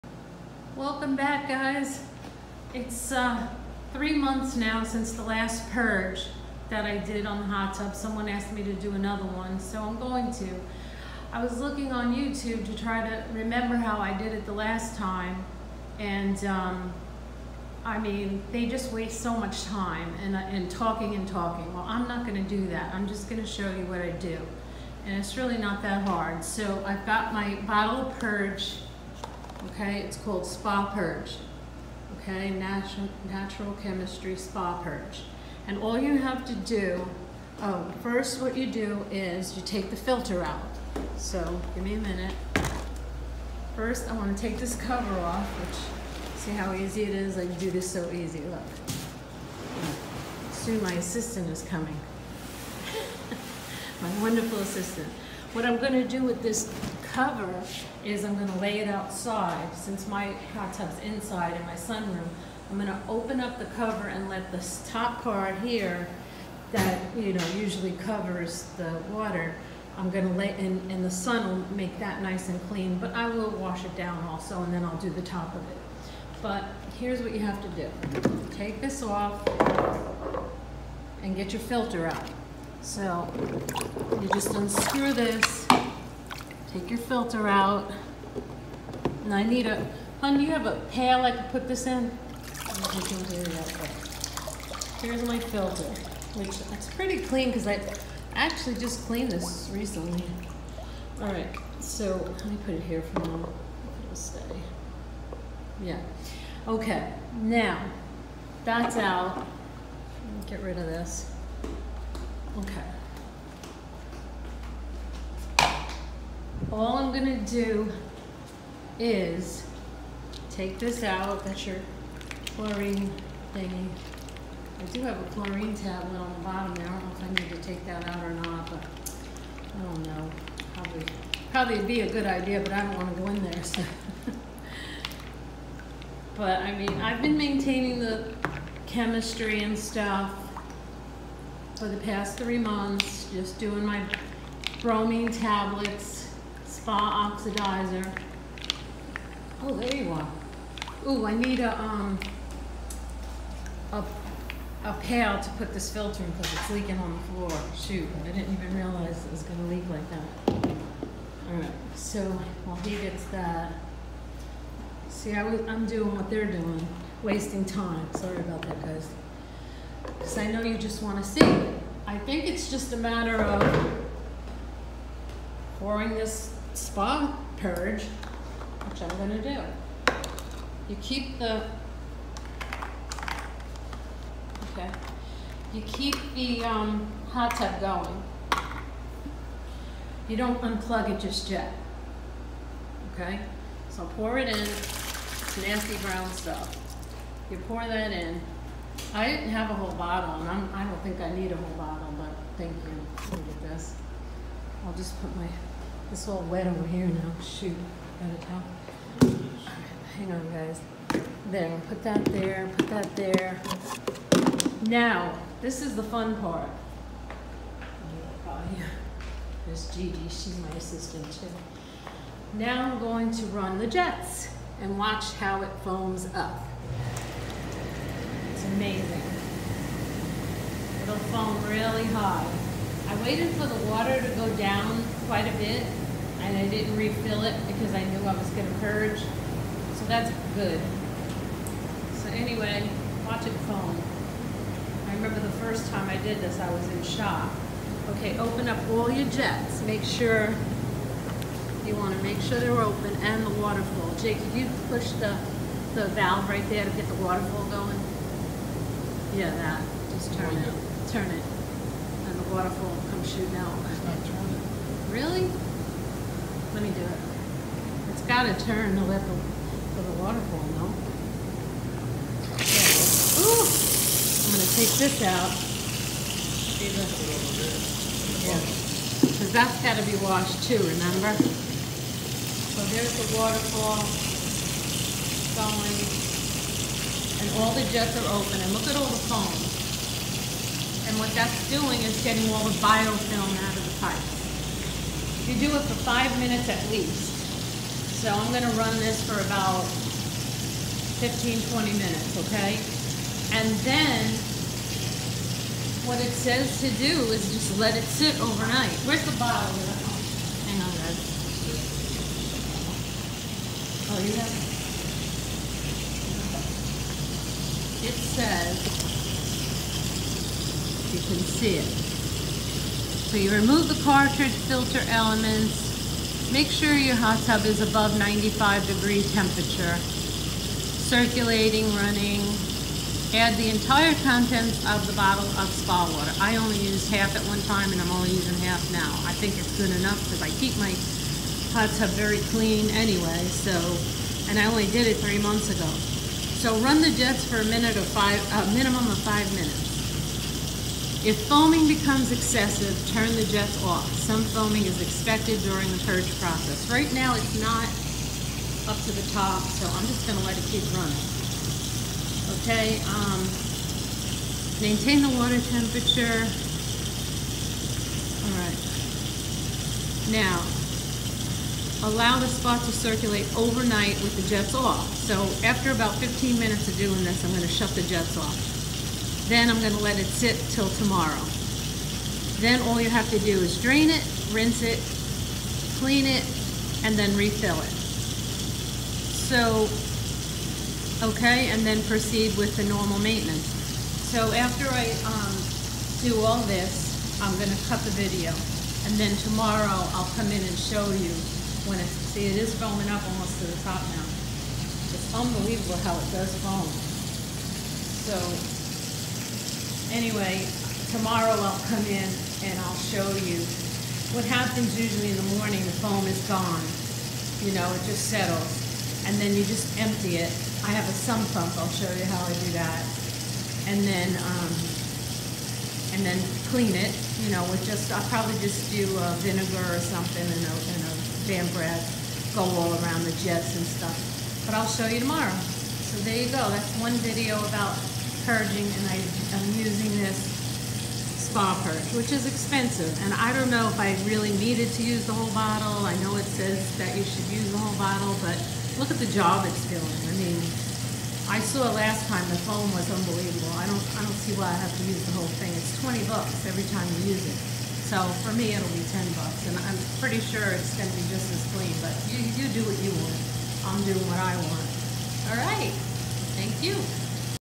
Time for Spa Purge! Aquarest 600.
Aquarest 600 Premium Spa purging another after 3 months. Short and to the point!